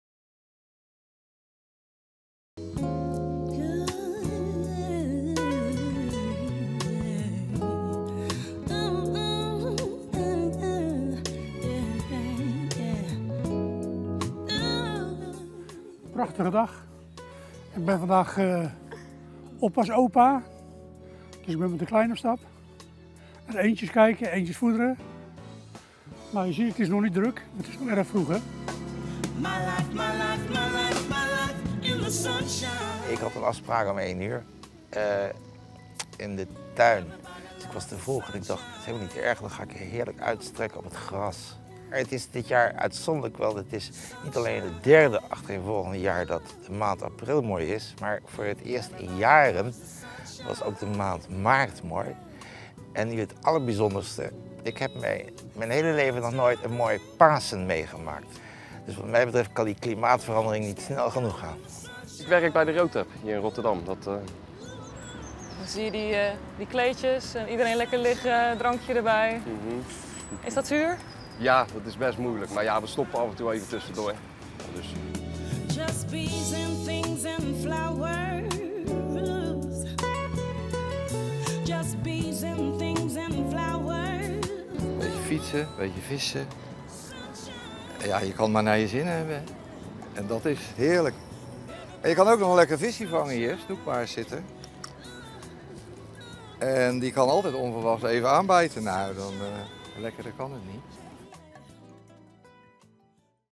Regio Rijnmond geniet van het mooie weer
Voor iedereen die verplicht binnen moest blijven voor school of werk is het misschien wat minder leuk, maar zij die een vrije dag hadden genoten met volle teugen van het prachtige lenteweer. Of alle records gebroken zijn, is nog niet bekend. In Hoek van Holland bereikten de temperaturen in elk geval zomerse waarden van 25 graden.